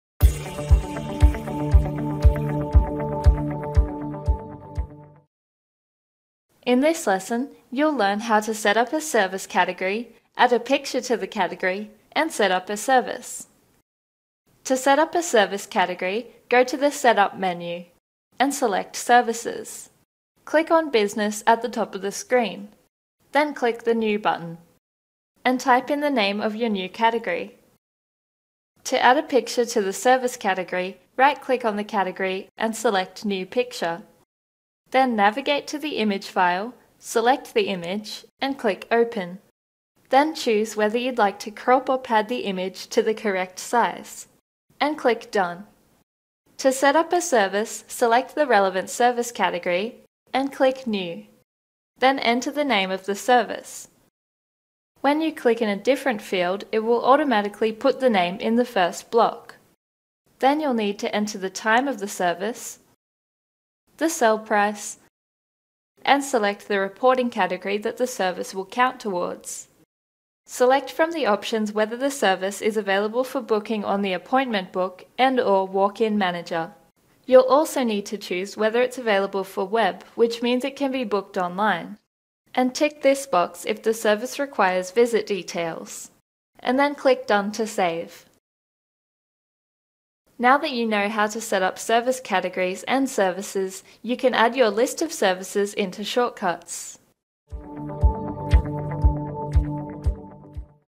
Shortcuts Software: Setting up Service Categories and Services
Learn how to set up your service categories with Shortcuts Software.